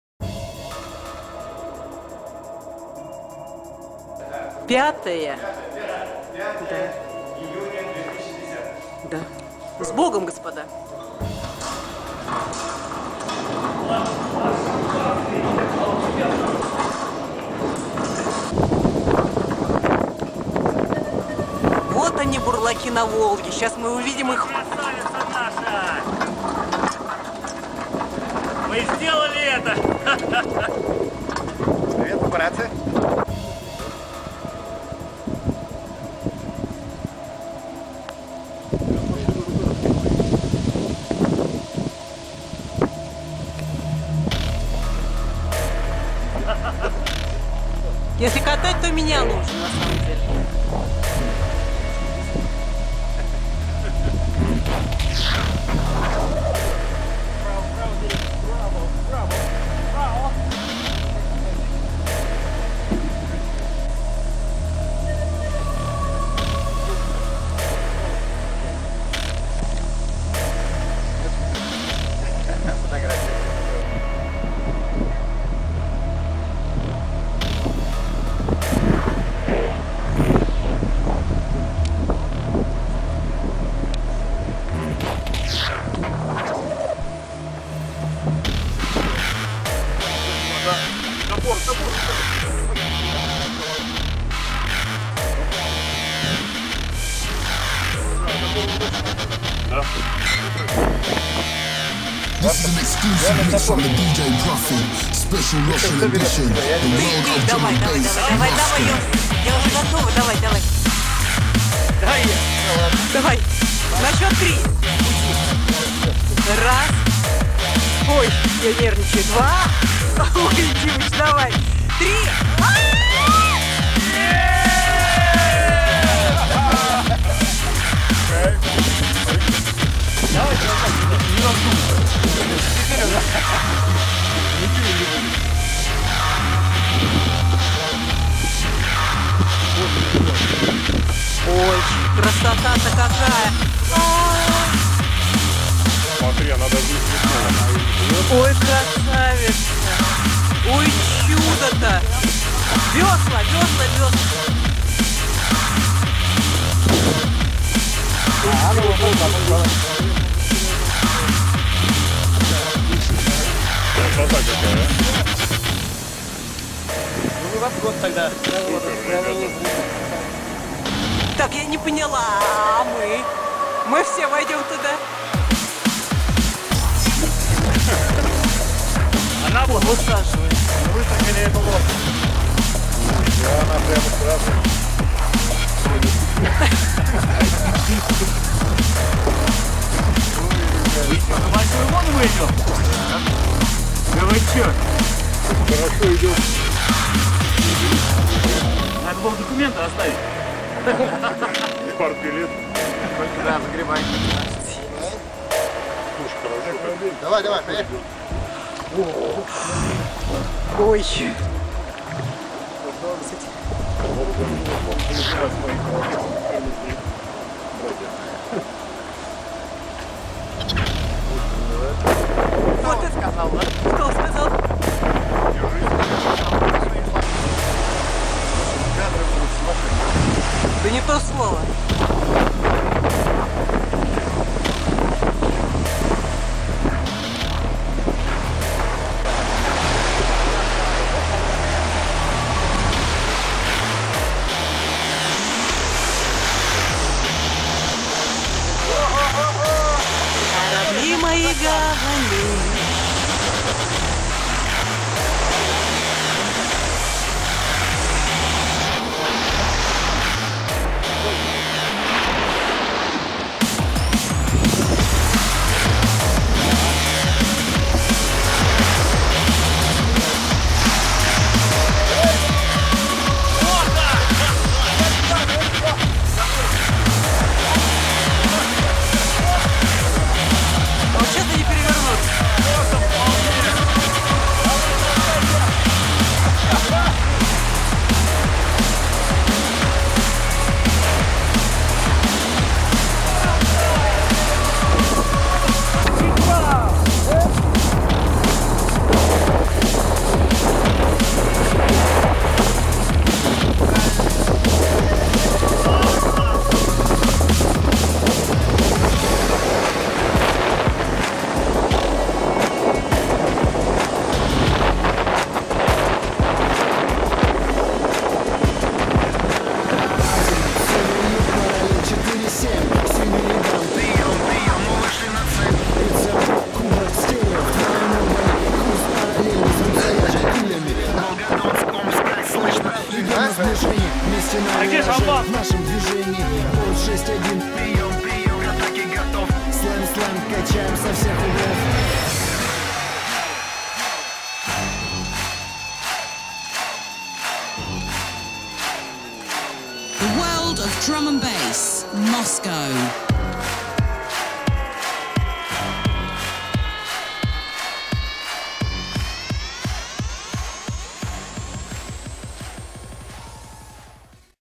Спуск на воду лодки, сделанной своими руками
5 июня ученики Школы Плотников "Тайга" торжественно спустили на воду лодку, сделанную своими руками за 10 дней под руководством Старейшего Мастера Плотницкого Искусства Юрия Григорьевича Спирина в Школе Плотников "Тайга". Пожелаем ученику школы Плотников Дмитрию из Фрязино 7 футов под килем его новой лодке!